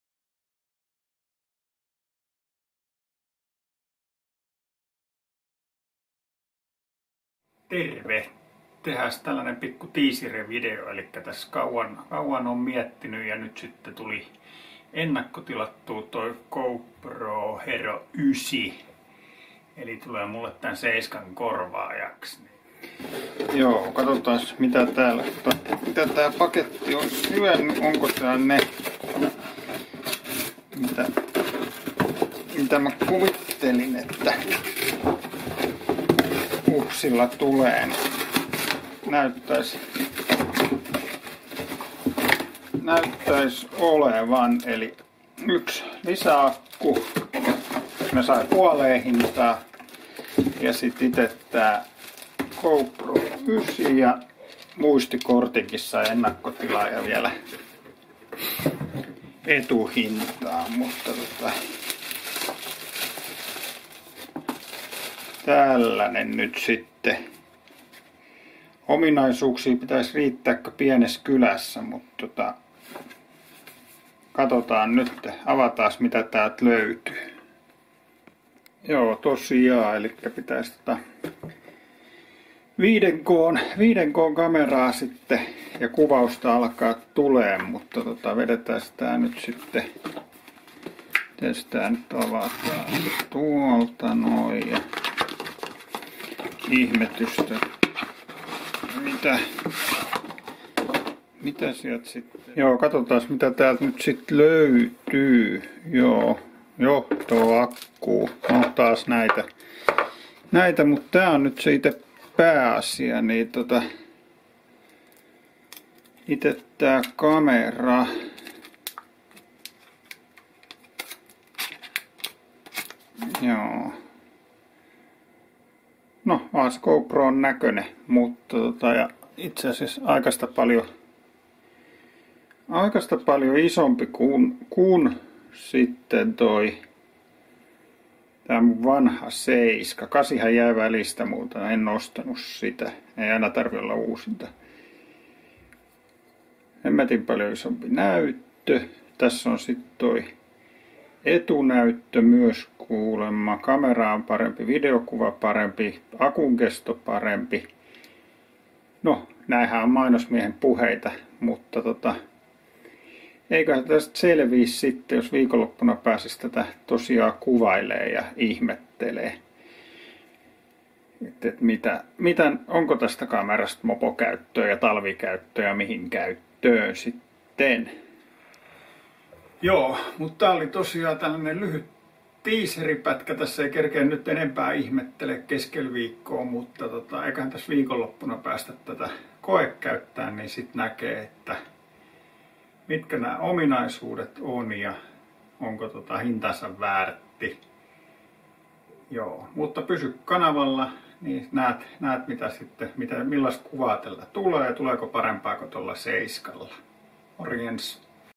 Uusi GoPro Hero9 lyhyt teaser
Lyhyt teaser ennakkoon, GoPro Hero9 maistiaiset, vanhan Hero7 tilalle uutta kameraa moporeissuille ja mitä sitä nyt sitten kuvaileekaan. Paketista pois ja lataukseen, ennen varsinaista testausta.